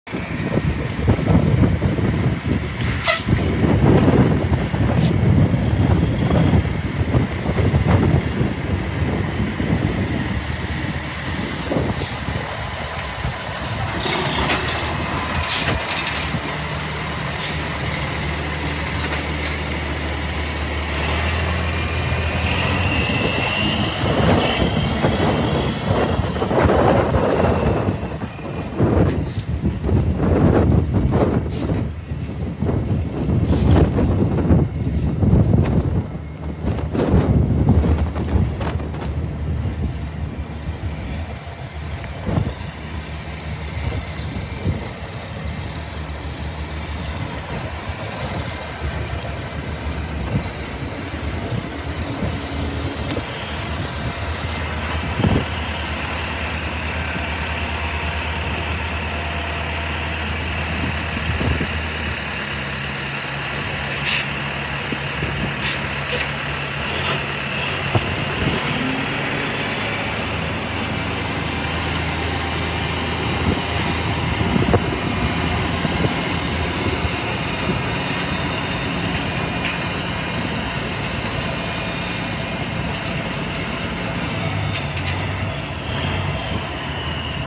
1990 FREIGHTLINER FL112 For Sale
Coming up for auction, 1990 Freightliner dump truck, Leverton Enterprises has inspected this unit and all systems appear to function as expected, please refer to the buyers terms and conditions prior to bidding, SOLD AS IS, WHERE IS, located in Rush Springs, OK.